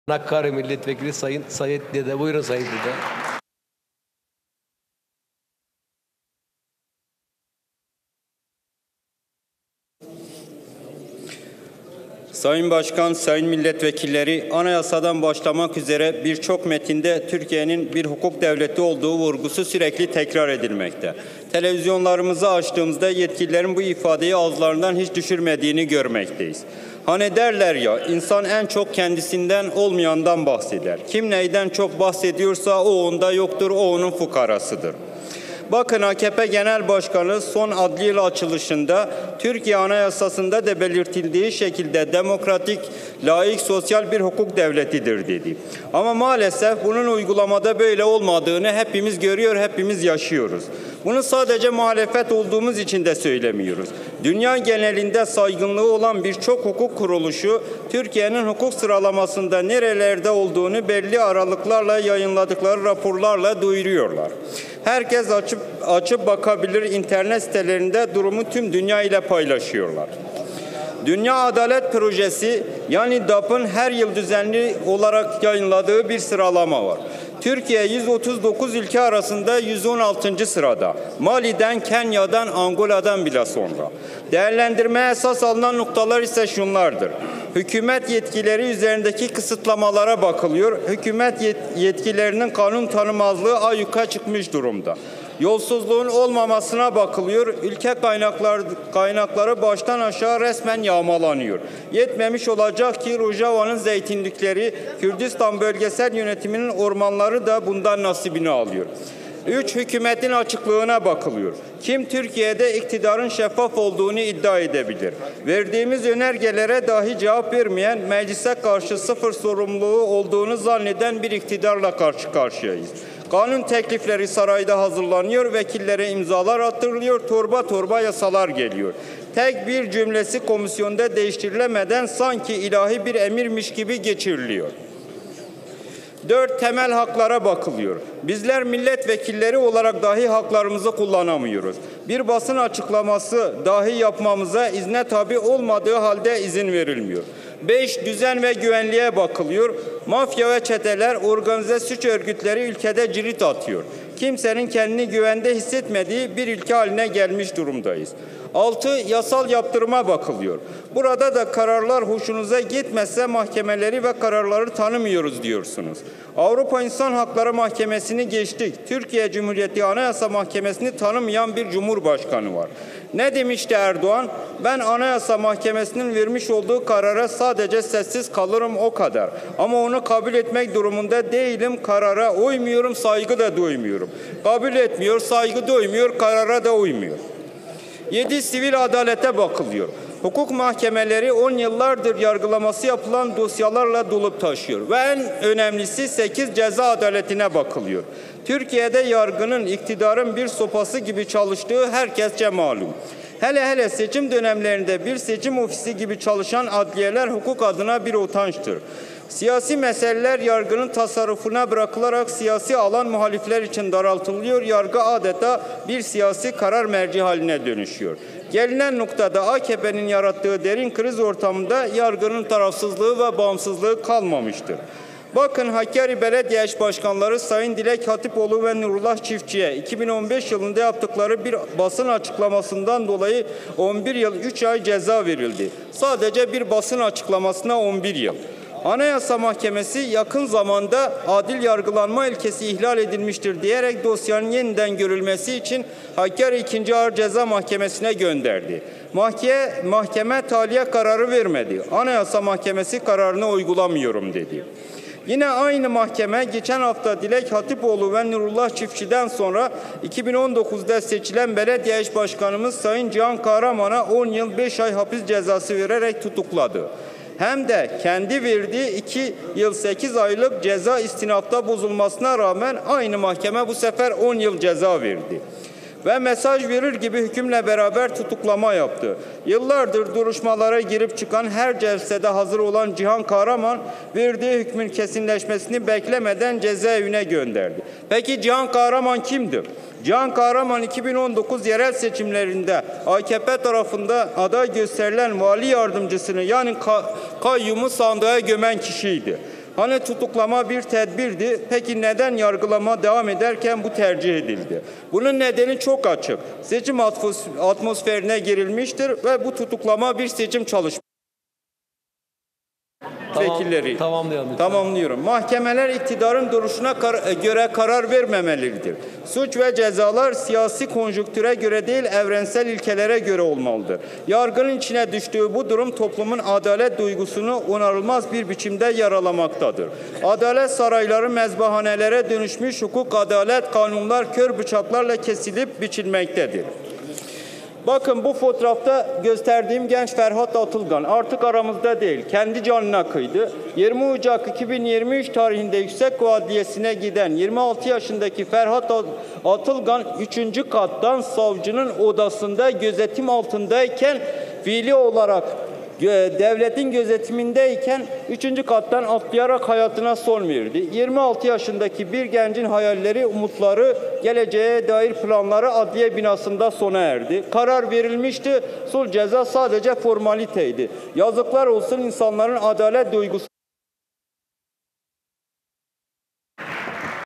Sait Dede Meclis konuşmasında Ferhat Atılgan'ı gündeme getirdi
HDP Hakkari Milletvekili Sait Dede, Türkiye Büyük Millet Meclisi'nde (TBMM) yaptığı konuşmada geçtiğimiz günlerde aldığı hapis cezası sonrası tutuklanarak cezaevine gönderilen Hakkari Belediyesi Eş Başkanı Cihan Kahraman ve Yüksekova'da Adliye binasından atlayarak intihar ettiği iddia edilen Ferhat Atılgan'ı gündeme getirdi.

Haberin detayları için internet sitemizi ziyaret edebilirsiniz.

Yüksekova Haber Portalı